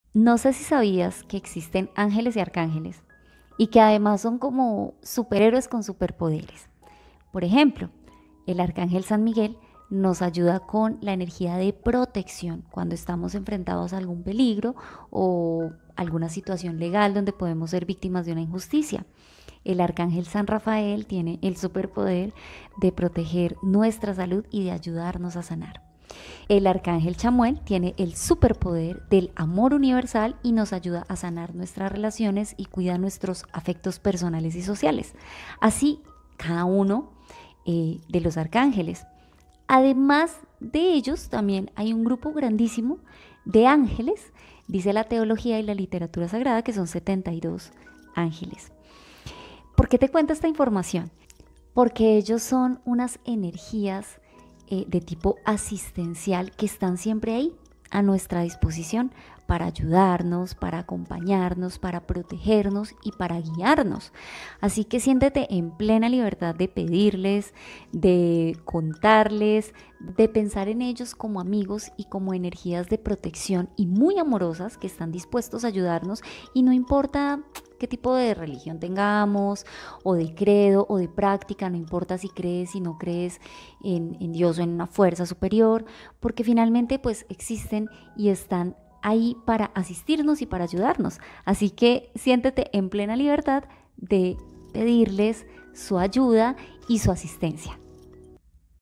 👉 existen los ÁNGELES Y ARCÁNGELES?
👼👼Son seres asistenciales
😀😀Aprende un par de cosas básicas de los guías asistenciales que llamamos ángeles.

Aprendamos juntos, sobre estos seres asistenciales,